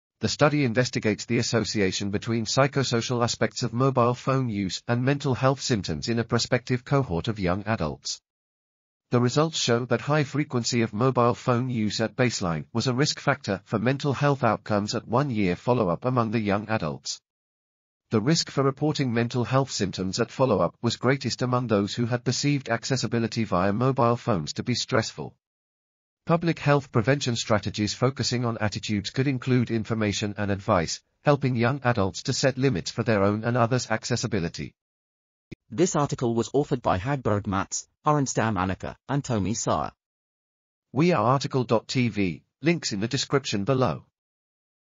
Mobile phone use and stress, sleep disturbances, and symptoms of depression among you... | RTCL.TV
### Keywords ###

### Article Attribution ###
Title: Mobile phone use and stress, sleep disturbances, and symptoms of depression among young adults - a prospective cohort study
Authors: Hagberg Mats, Härenstam Annika ,and Thomée Sara
Publisher: BMC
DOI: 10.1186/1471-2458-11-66

### Video Timestamps ###
0:00:00 - Summary
0:00:35 - Title
0:00:40 - Tail
0:00:44 - End